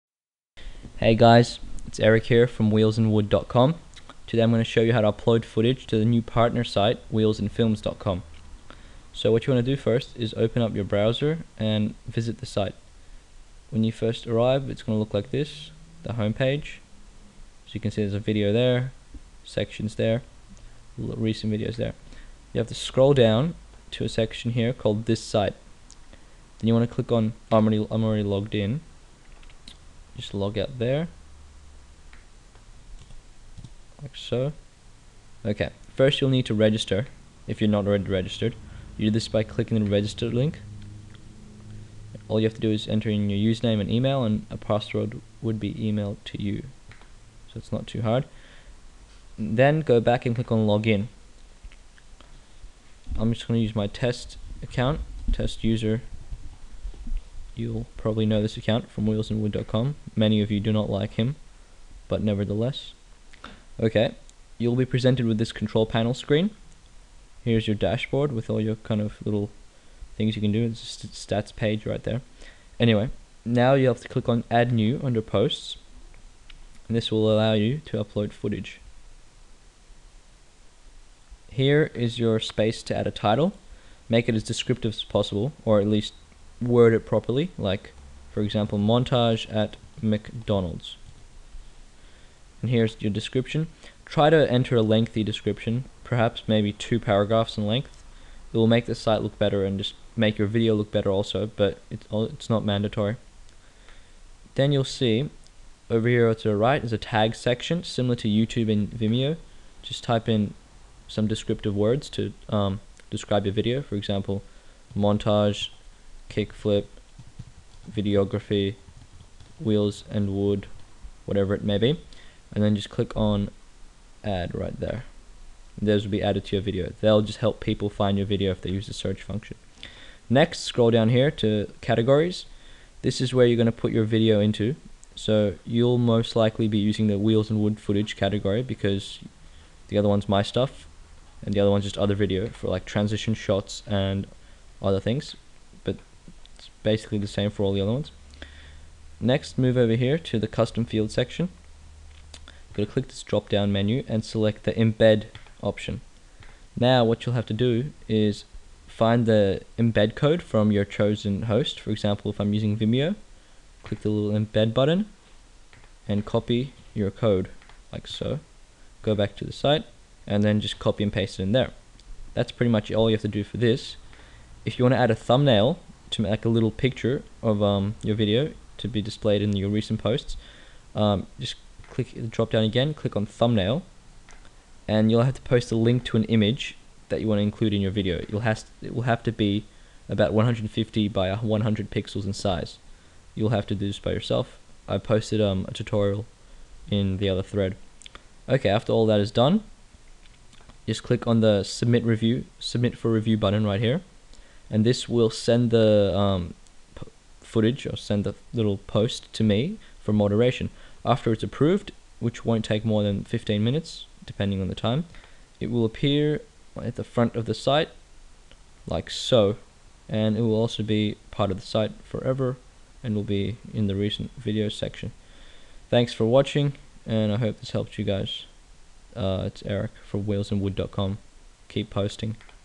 Wheels In Films Footage Upload Tutorial (wheelsandwood.com partner site)
The tutorial on how to upload videos to wheelsinfilms.com